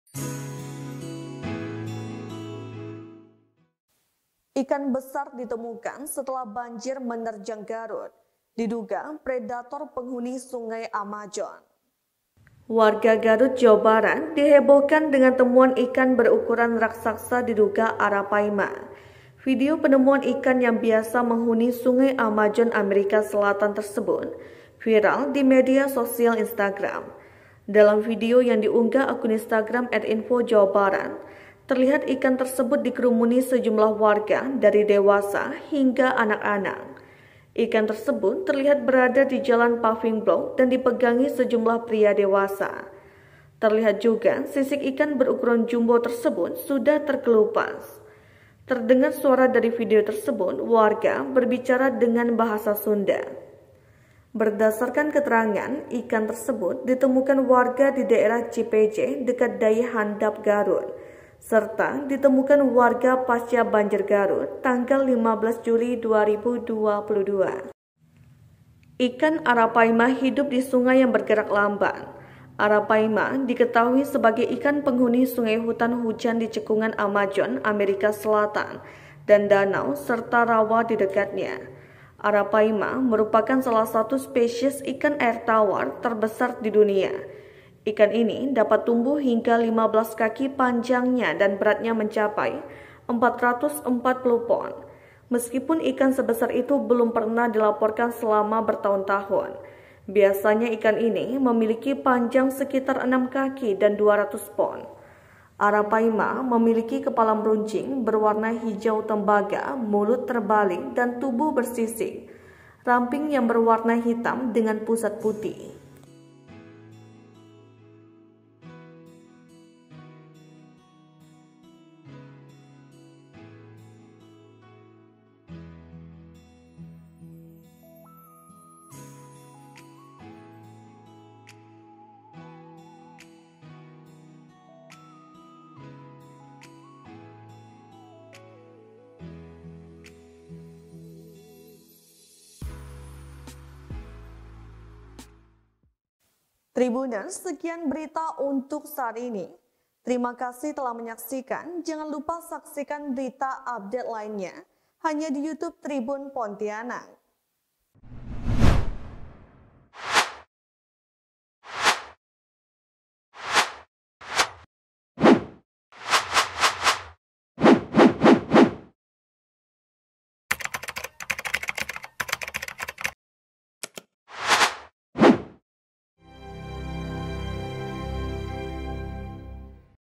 🔴 Ikan Berukuran Besar Ditemukan Setelah Banjir Garut, Diduga Arapaima Predator Sungai Amazon
Warga Garut, Jawa Barat dihebohkan dengan temuan ikan berukuran raksasa diduga Arapaima.

Dalam video yang diunggah akun instagram @infojawabarat, terlihat ikan tersebut dikerumuni sejumlah warga dari dewasa hingga anak-anak.